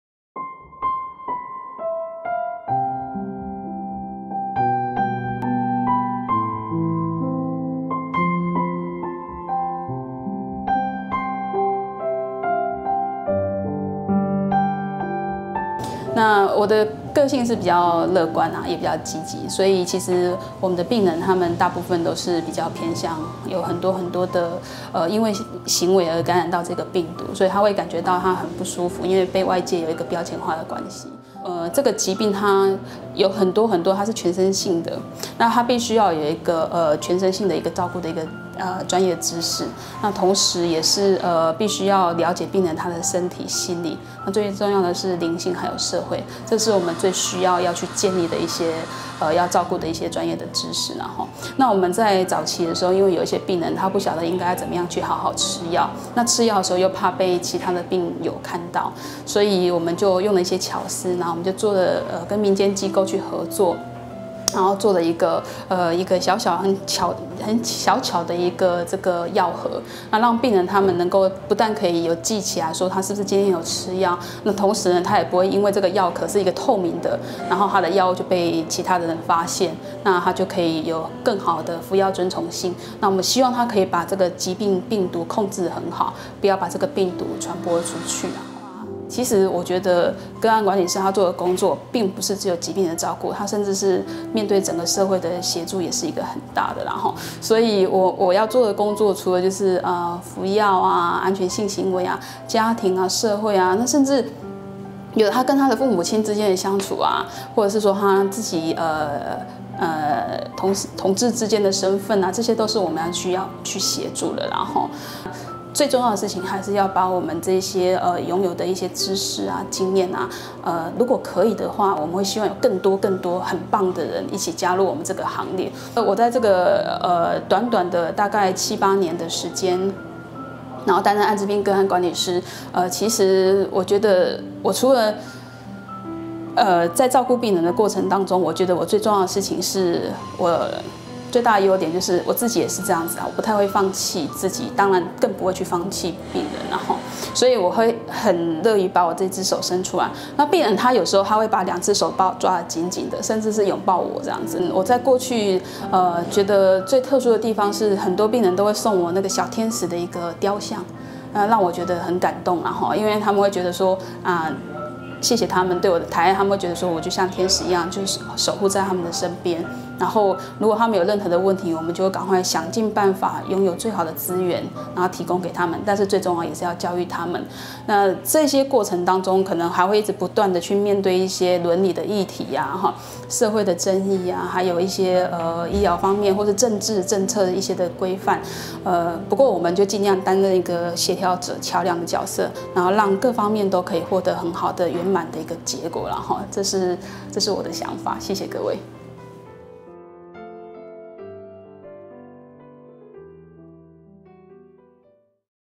【2015醫療心職人】愛的滋味~守護愛滋個管的小巨人 謝佳吟個管師
財團法人醫院評鑑暨醫療品質策進會 醫療心職人
愛的滋味~守護愛滋個管的小巨人 台中榮民總醫院  謝佳吟個管師

謝個管師主要擔任台中榮總愛滋病個案管理師，
由於愛滋病人服藥順從低，
因此設計感染者願意使用的藥盒，
並協助愛滋病人服藥、安全性行為、倫理、社會等議題，
除了照顧病人外，最重要的是不會放棄病人。

地        址：220新北市板橋區三民路二段31號5F